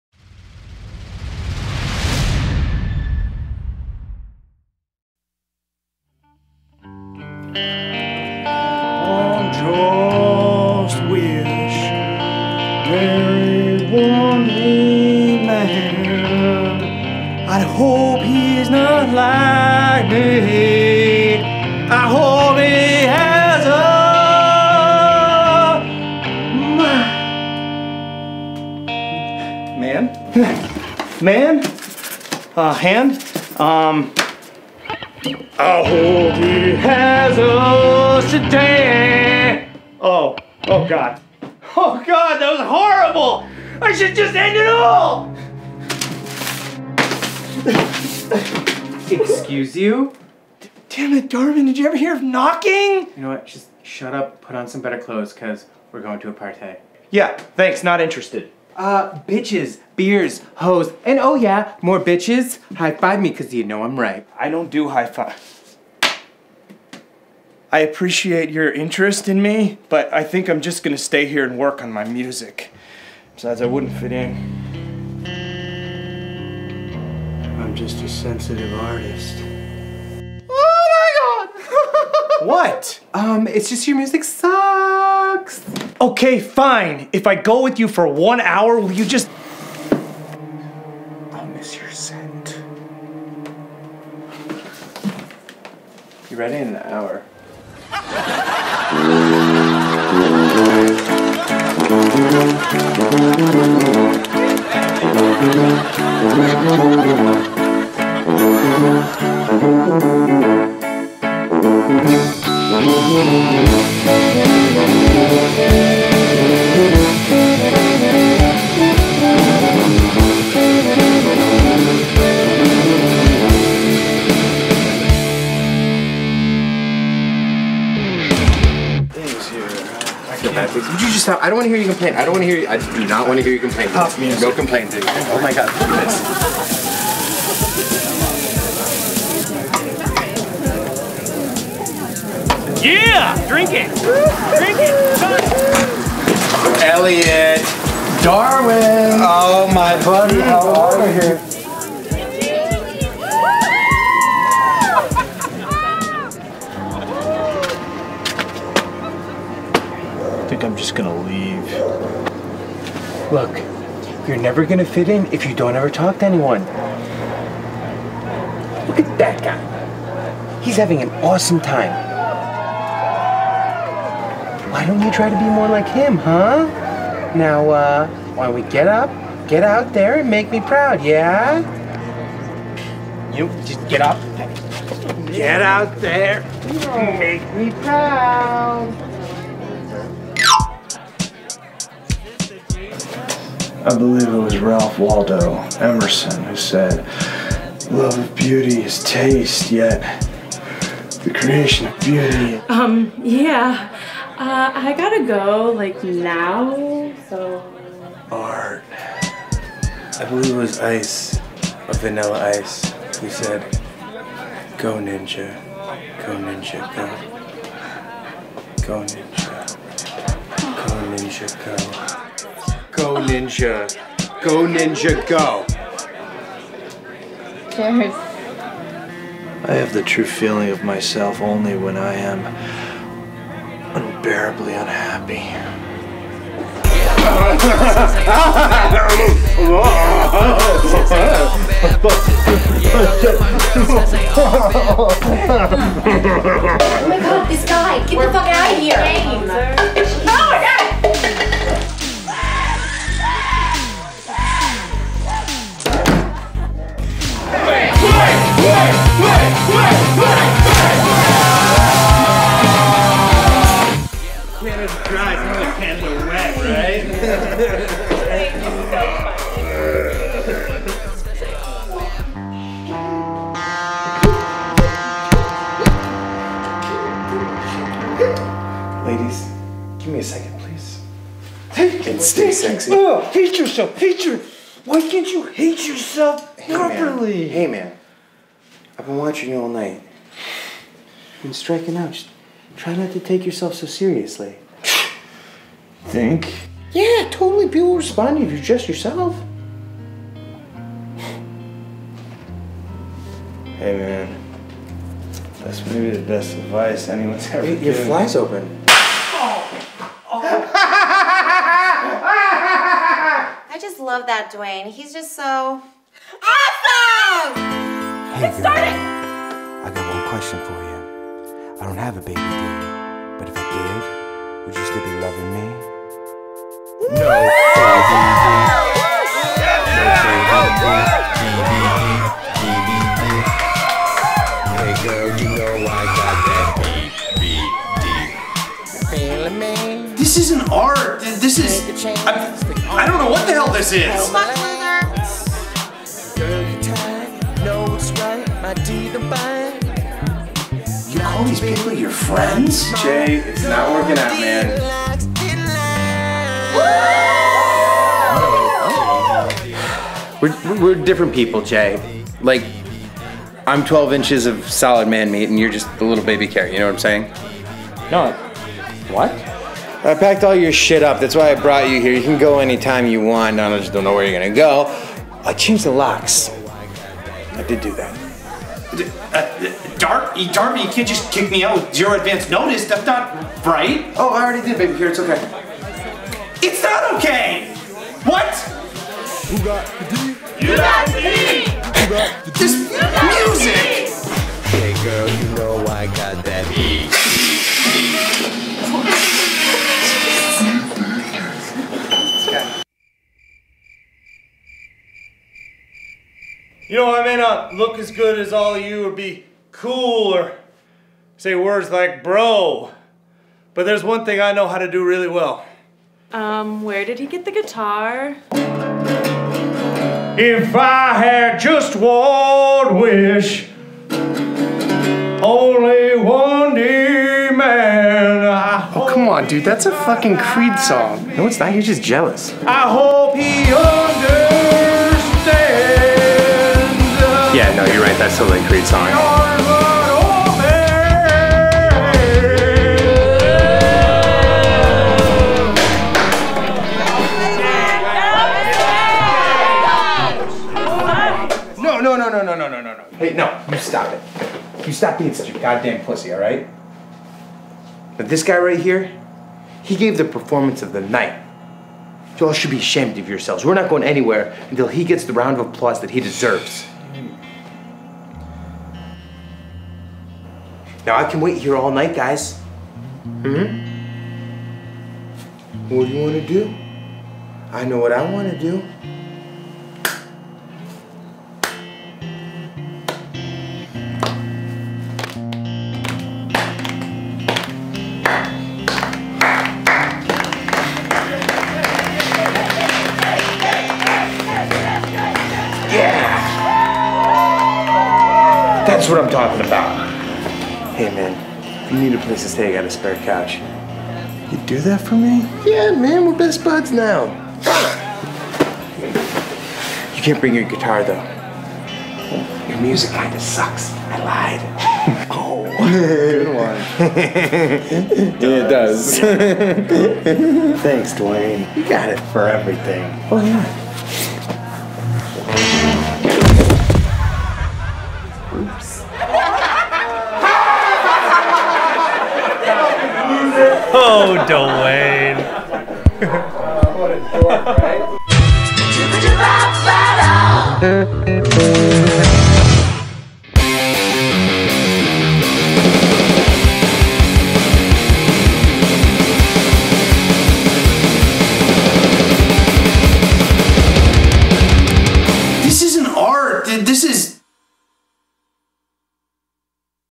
Episode Six: Jay and Dwayne Meet Cute
It’s the late 90s and Jay, a wild-haired Eddie Vedder clone, is a sensitive young man who’s shy and reserved, spending most of his days locked away in his room, writing sad music no one will ever listen to. Jay’s roommate, a frosty-haired rogue named Darvin, invites Jay to a party, where he casually tells Jay that he’s kicking him out. Now, feeling isolated and with no place to live, Jay will have to rely on a certain charismatic boy band leader, corn crop haircut and all, to show him how to be normal and carefree. That is if Jay doesn’t completely embarrass himself first. This is the origin story of the greatest Bromance of all time: the Jay and Dwayne saga.